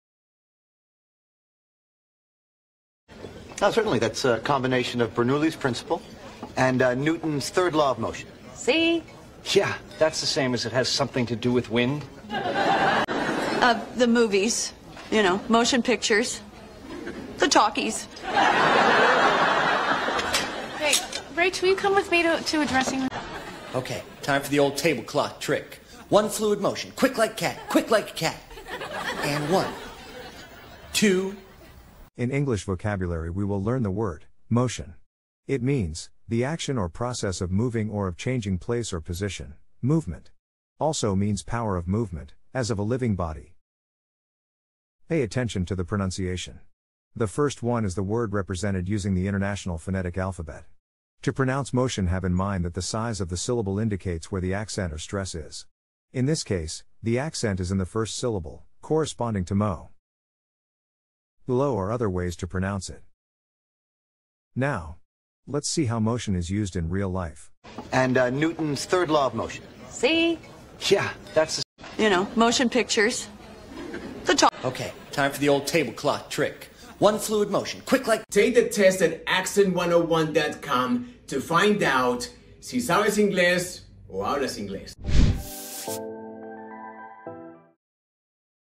Vocabulario en inglés basico: 'MOTION'
En esta Clase de inglés aprenderemos la palabra MOTION (movimiento) con ejemplos de series de televisión

MOTION significa:

- The action or process of moving or of changing place or position; movement.
- Power of movement, as of a living body.
- The manner of moving the body in walking; gait.
- A bodily movement or change of posture; gesture.
- A proposal formally made to a deliberative assembly: to make a motion to adjourn.
-  Law.
- An application made to a court or judge for an order, ruling, or the like.
- A suggestion or proposal.
- An inward prompting or impulse; inclination: He will go only of his own motion.
-  Music.
- Melodic progression, as the change of a voice part from one pitch to another.
-  Machinery.
- A piece of mechanism with a particular action or function.
- The action of such a mechanism.
- To direct by a significant motion or gesture, as with the hand: to motion a person to a seat.
- To make a meaningful motion, as with the hand; gesture; signal: to motion to someone to come.
-  go through the motions,
- To do something halfheartedly, routinely, or as a formality or façade.
-  in motion,
- In active operation; moving: The train was already in motion when he tried to board it.
- The process of continual change in the physical position of an object; movement
- A movement or action, esp of part of the human body; a gesture
- The capacity for movement
- A manner of movement, esp walking; gait
- A mental impulse
- A formal proposal to be discussed and voted on in a debate, meeting, etc
- Law
- An application made to a judge or court for an order or ruling necessary to the conduct of legal proceedings
- British
- The evacuation of the bowels
- Excrement
- Part of a moving mechanism
- The action of such a part
- Music
- The upward or downward course followed by a part or melody. Parts whose progressions are in the same direction exhibit similar motion, while two parts whose progressions are in opposite directions exhibit contrary motion
- To act or perform the task (of doing something) mechanically or without sincerity
- To mimic the action (of something) by gesture
- In motion
- Operational or functioning (often in the phrases set in motion, set the wheels in motion)
- (when tr, may take a clause as object or an infinitive)
- To signal or direct (a person) by a movement or gesture
- Sir Andrew. born 1952, British poet and biographer; his collections include Pleasure Steamers (1978) and Public Property (2002): poet laureate (1999–2009)
- The act or process of changing position or place.
- The manner in which the body or a body part moves.

Let's see how 'MOTION' is used in Advanced English vocabulary:

- to make a motion to adjourn. : para hacer una moción para levantar la sesión.
- He will go only of his own motion. : Irá solo por su propio movimiento.
- to motion a person to a seat. : para indicarle a una persona que se siente.
- to motion to someone to come. : para indicarle a alguien que venga.
- The train was already in motion when he tried to board it. : El tren ya estaba en movimiento cuando intentó abordarlo.

Para aprender es importante saber el vocabulario en inglés con promunciación, MOTION se pronuncia ˈmoʊʃən en el IPA o [ moh-shuh n ]